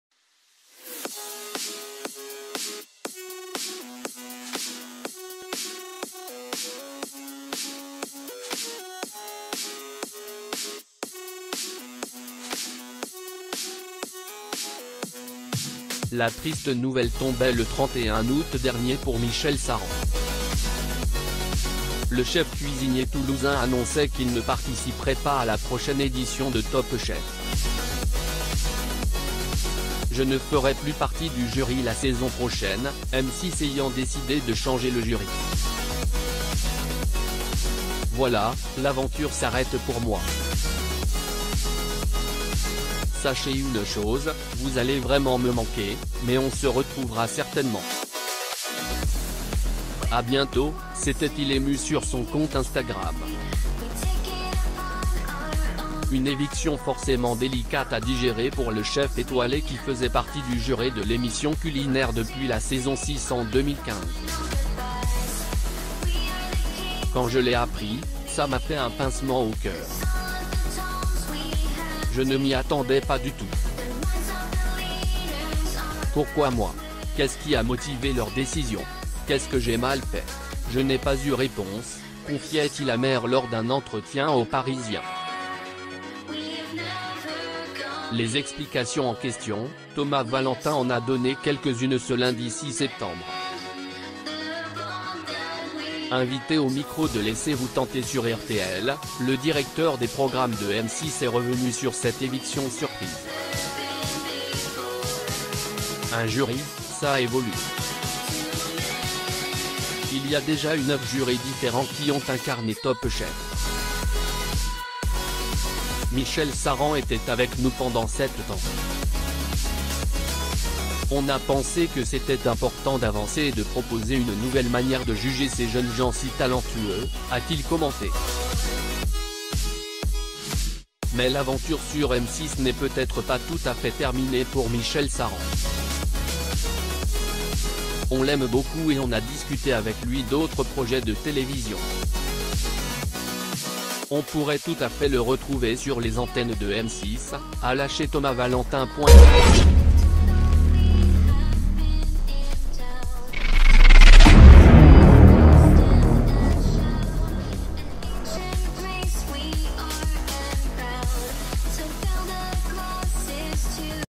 Michel Sarran viré de Top chef : le directeur des programmes de M6 s’explique
,michel sarran viré de top chef il fait une mise au point : “je ne fais pas de...,une,fait,michel sarran,
Le chef cuisinier toulousain annonçait qu'il ne participerait pas à la prochaine édition de "Top Chef". "Je ne ferai plus partie du jury la saison prochaine, M6 ayant décidé de changer le jury. Voilà, l'aventure s'arrête pour moi. Sachez une chose, vous allez vraiment me manquer, mais on se retrouvera certainement. À bientôt", s'était-il ému sur son compte Instagram. Une éviction forcément délicate à digérer pour le chef étoilé qui faisait partie du juré de l'émission culinaire depuis la saison 6 en 2015. "Quand je l'ai appris, ça m'a fait un pincement au cœur. Ça a été un peu violent. Je ne m'y attendais pas du tout. Pourquoi moi ? Qu'est-ce qui a motivé leur décision ? Qu'est-ce que j'ai mal fait ? Je n'ai pas eu réponse", confiait-il amer lors d'un entretien au Parisien. Les explicat